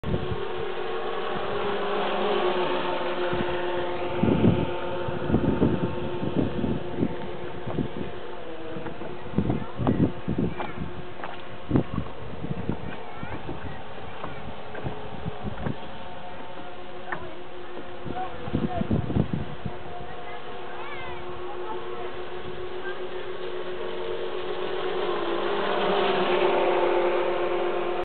Motorboot Moonraker, ferngesteuertes Modellboot
Schnelles, ferngesteuertes Modell-Motorboot 1,48 lang